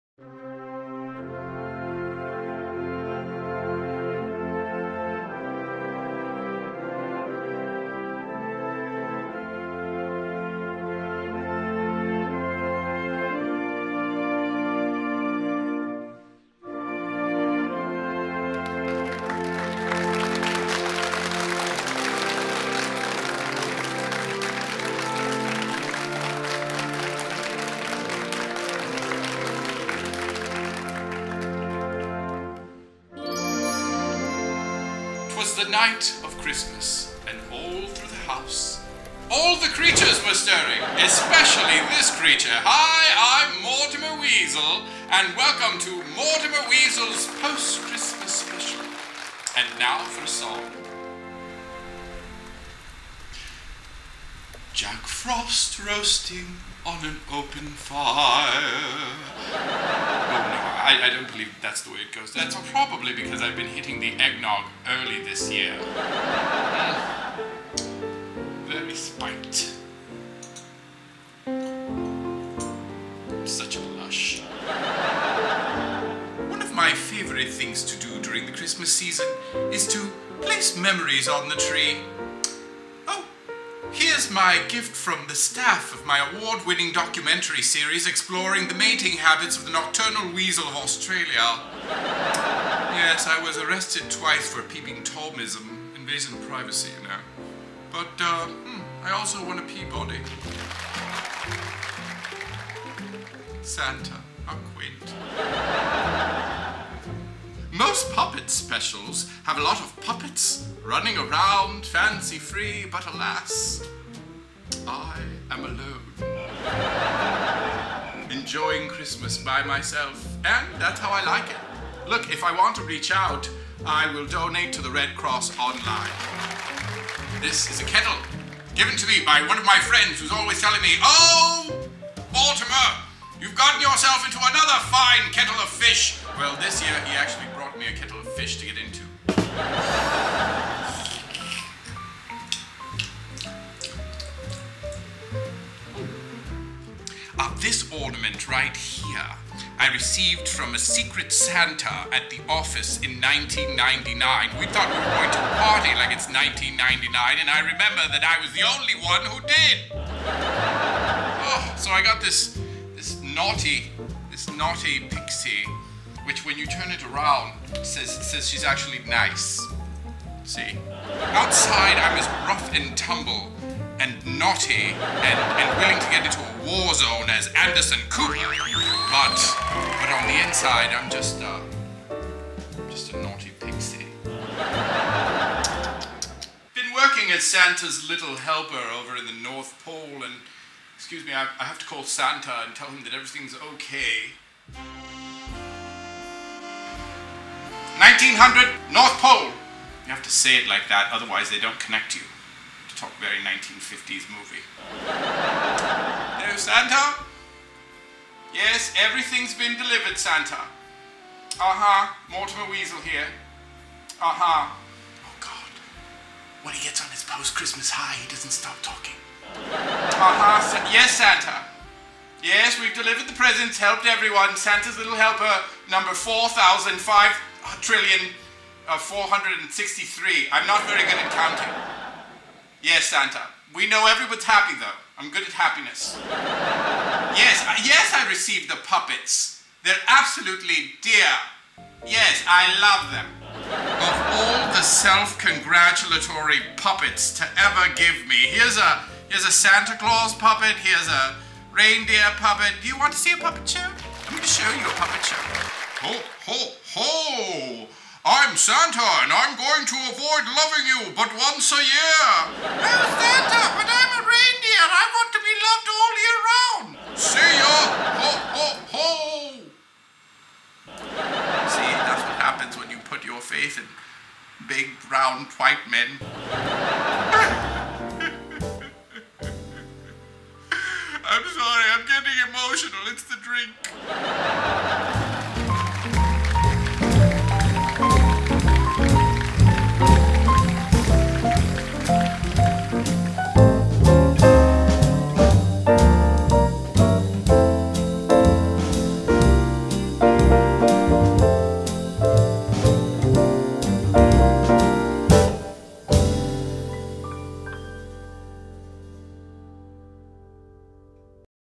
THE MORTIMER WEASEL POST-CHRISTMAS HOLIDAY SPECIAL
Mortimer Weasel brings you a delightful Christmas Special, filled with reminiscences, a call to Santa and his own puppet show. Be enchanted by downloading the exclusive, FREE LosTiteres.TV Holiday Ornament at: ‪